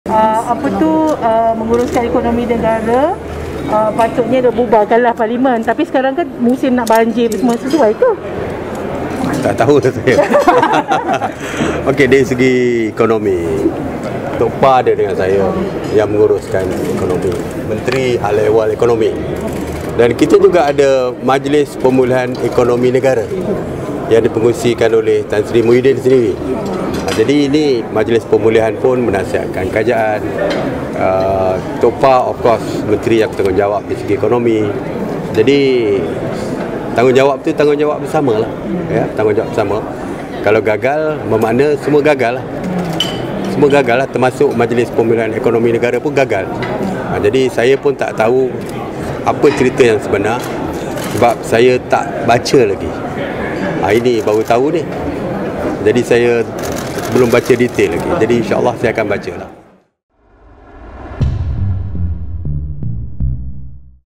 Kalau kerajaan gagal, maksudnya Tok Pa, Muhyiddin juga gagal - PM
Selepas disindir Muhyiddin Yassin, Perdana Menteri Ismail Sabri Yaakob berkata jika benar kerajaan gagal menguruskan ekonomi seperti didakwa, Bersatu turut 'menyumbang' kepada situasi tersebut.

Ismail Sabri mengingatkan dalam pentadbirannya, ada beberapa pemimpin Bersatu turut menganggotainya seperti Mustapa Mohamed serta Muhyiddin yang mempengerusikan Majlis Pemulihan Negara.